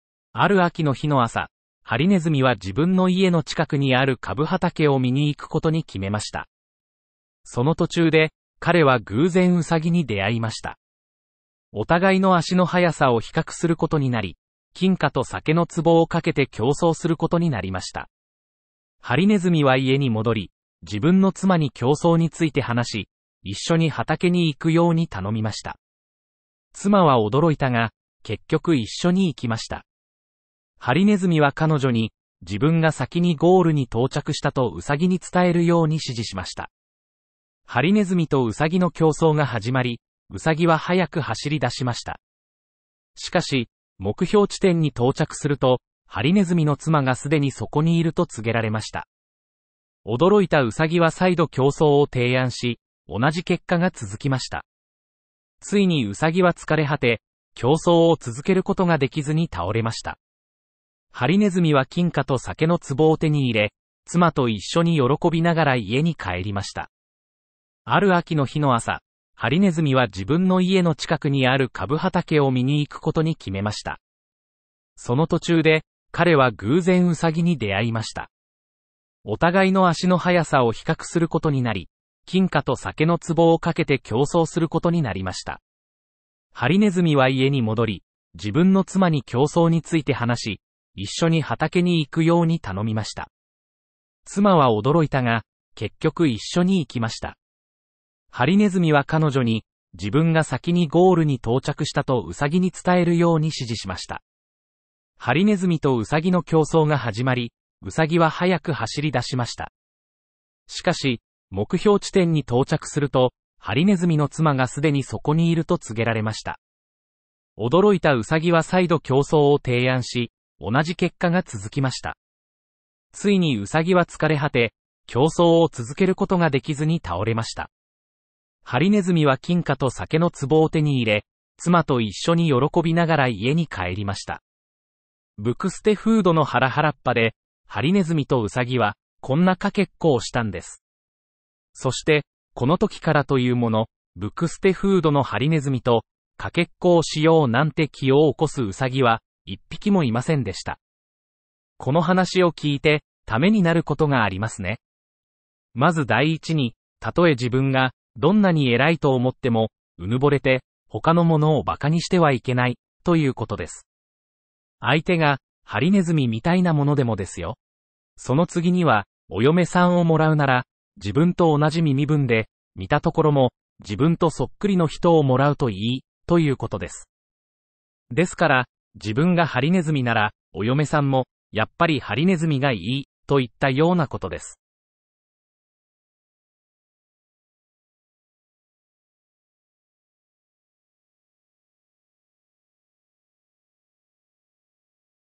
うさぎとハリネズミ Rabbit and Hedgehog
ある秋の日の朝、はりねずみは自分の家の近くにあるかぶ畑を見に行くことに決めました。その途中で、彼は偶然うさぎに出会いました。お互いの足の速さを比較することになり、金貨と酒の壺を賭けて競争することになりました。

はりねずみは家に戻り、自分の妻に競争について話し、一緒に畑に行くように頼みました。妻は驚いたが、結局一緒に行きました。はりねずみは彼女に、自分が先にゴールに到着したとうさぎに伝えるように指示しました。

はりねずみとうさぎの競争が始まり、うさぎは速く走り出しました。しかし、目標地点に到着すると、はりねずみの妻がすでにそこにいると告げられました。驚いたうさぎは再度競争を提案し、同じ結果が続きました。

ついにうさぎは疲れ果て、競争を続けることができずに倒れました。はりねずみは金貨と酒の壺を手に入れ、妻と一緒に喜びながら家に帰りました。

ブクステフードの原はらっぱで、はりねずみとうさぎは、こんなかけっこをしたんです。そして、このときからというもの、ブクステフードのはりねずみと、かけっこをしようなんて　気をおこすうさぎは、一ぴきもいませんでした。
　このはなしをきいて、ためになることがありますね。
　まず　だい一に、たとえ　じぶんが、どんなに　えらいとおもっても、うぬぼれて、ほかのものを　ばかにしてはいけない、ということです。あいてが、はりねずみみたいなものでもですよ。
　そのつぎには、およめさんをもらうなら、じぶんとおなじ身みぶんで、みたところも、じぶんとそっくりの人を　もらうといい、ということです。ですから、じぶんがはりねずみなら、およめさんも、やっぱり　はりねずみがいい、といったようなことです。